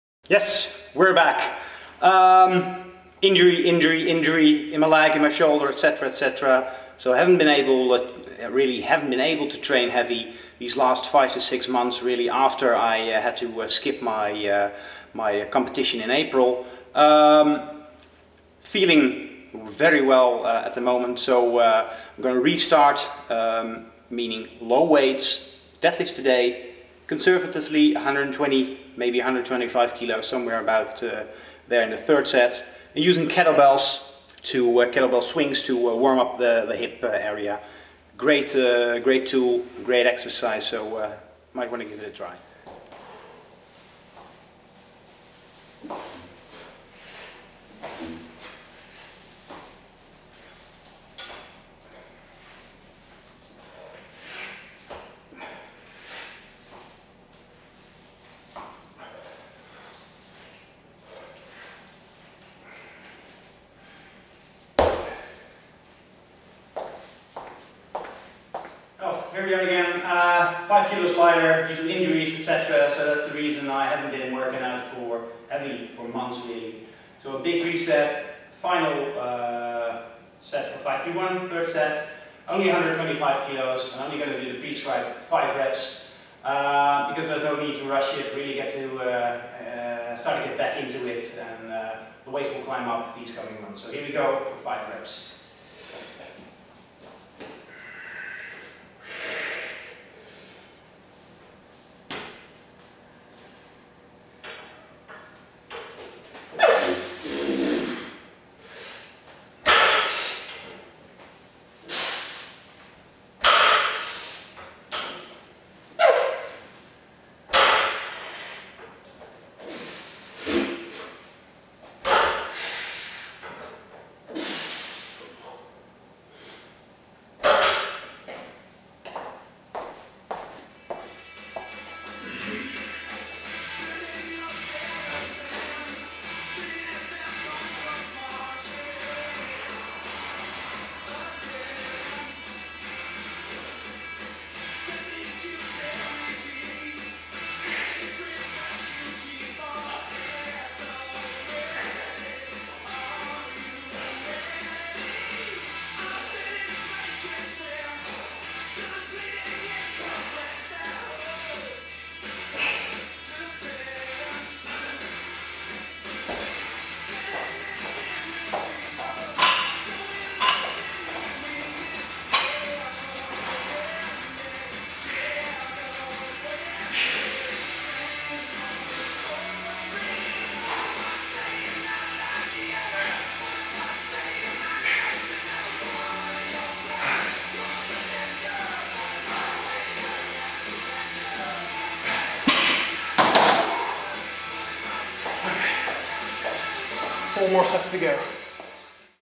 Wendler 531 restart after injury | light deadlifts and KB swings
First dead lift workout in 3 months and before that I didn't dead lift for 2 months. Same goes for squats and bench. So bodyweight and strength are way down, and these coming weeks my focus is to get the feel for the exercises back. The weights (and bodyweight) will go up as time progresses.